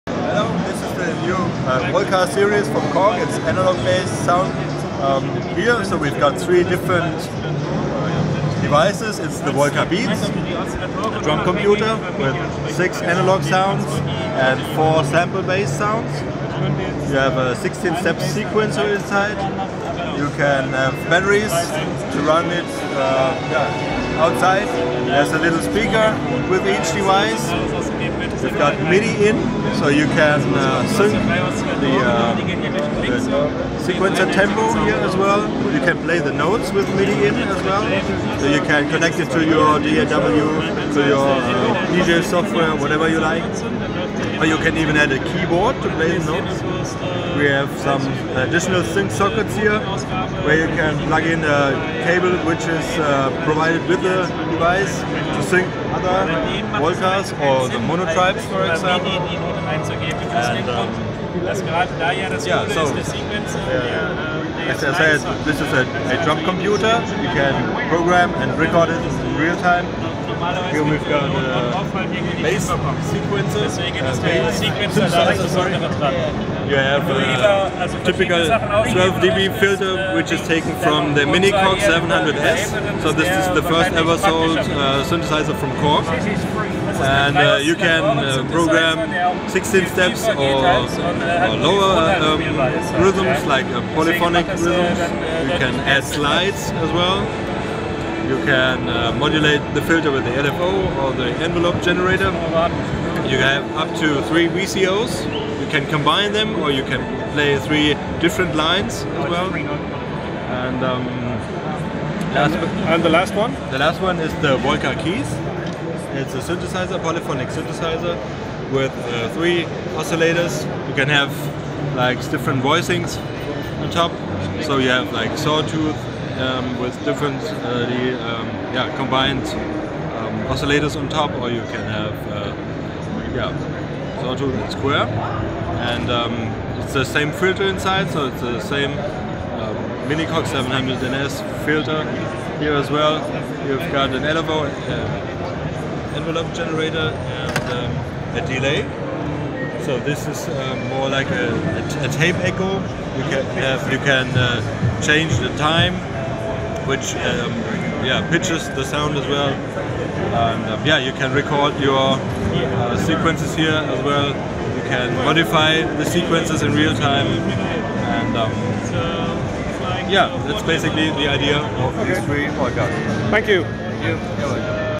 Stereoklang TV: Close look at KORG's new Volca boxes
The three new groove boxes from KORG - the Volca boxes -  featured at MusikMesse
Run through of the capabilities and features

Steelberry Clones at MusikMesse
Copyright Stereoklang Produktion